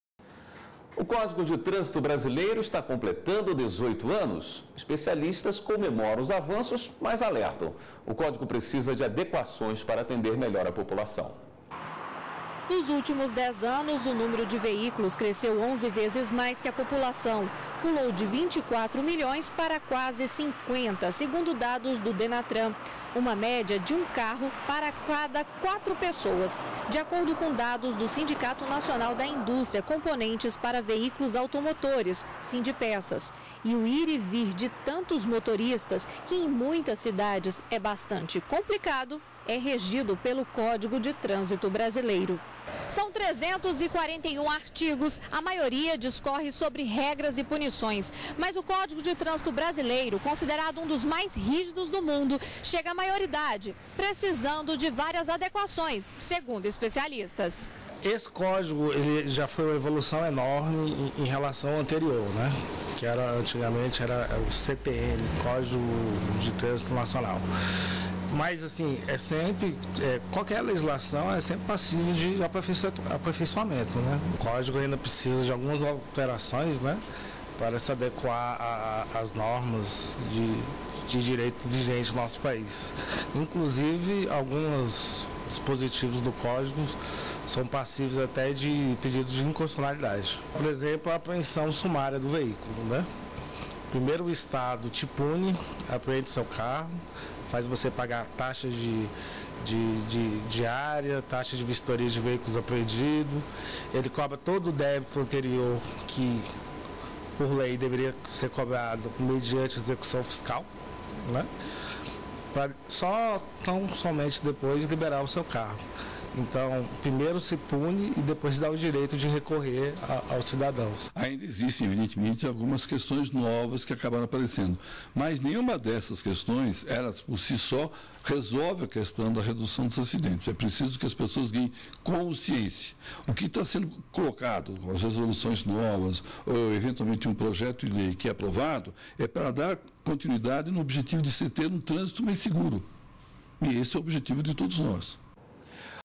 Código Brasileiro de Trânsito (23/09/15)
O Código de Trânsito está completando 18 anos. Especialistas comemoram os avanços, mas alertam: o Código precisa de adequações para atender melhor a população.